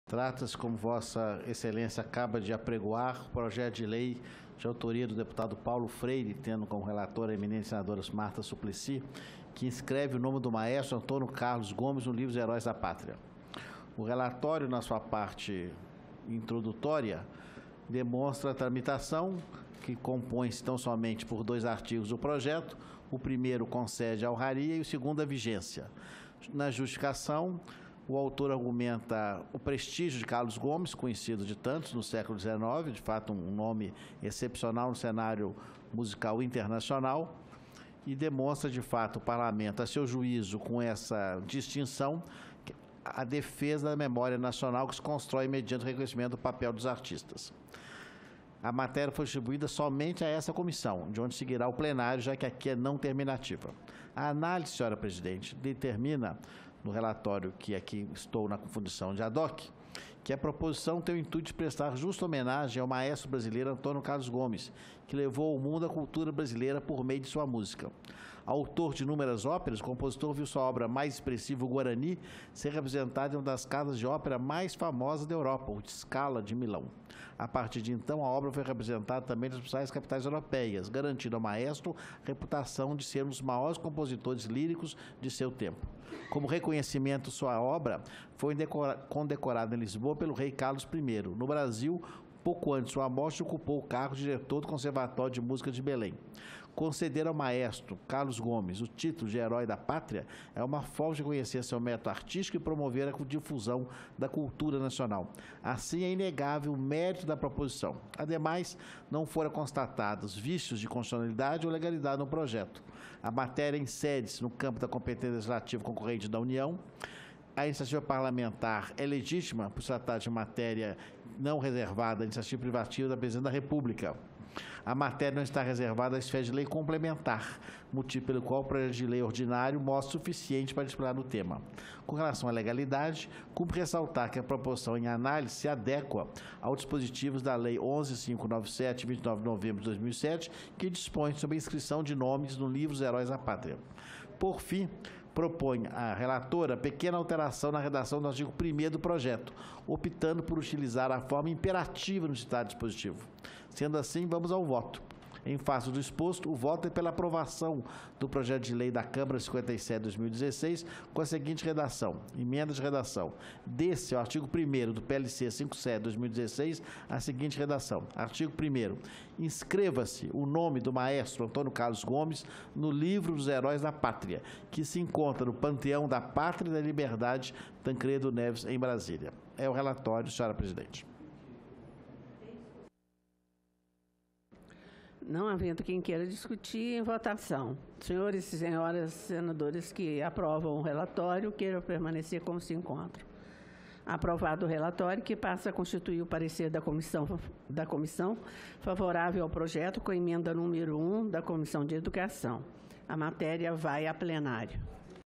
CE aprova inscrição do nome do maestro Antônio Carlos Gomes no Livro dos Heróis da Pátria
02/05/2017 - CE - CE aprova inscrição do nome do maestro Antônio Carlos Gomes no Livro dos Heróis da Pátria